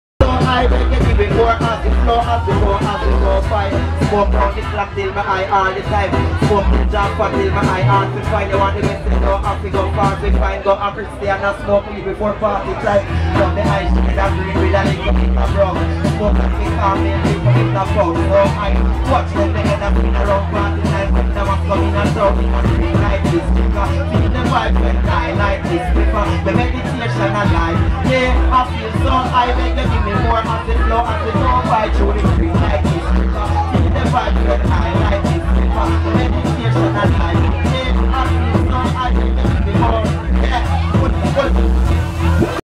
Damas at Donkey Club in Copenhagen
Damas w sound system at Donkey Club 23 Nov 2013. Damas showed the Danish audience what he could do dancehall style together with the Youngblood Sound crew